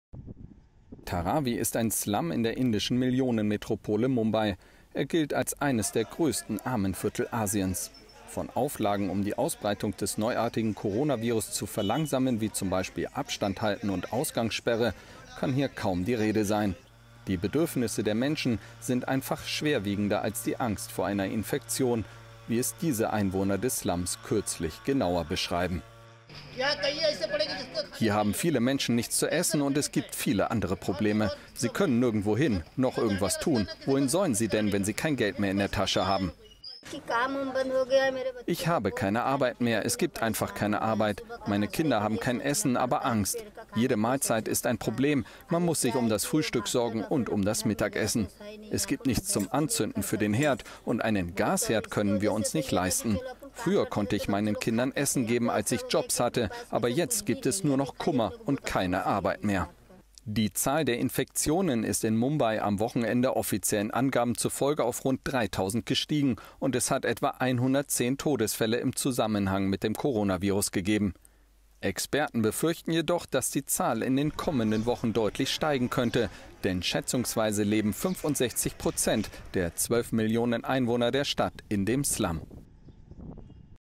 Deutlicher Anstieg von Covid-19-Fällen in indischem Slum befürchtet
In Mumbais Viertel Dharavi leben schätzungsweise fast acht Millionen Menschen in ärmlichsten Verhältnissen. Der Kampf gegen das Coronavirus ist dort stark erschwert. Mumbai meldete bisher rund 3000 Infektionen.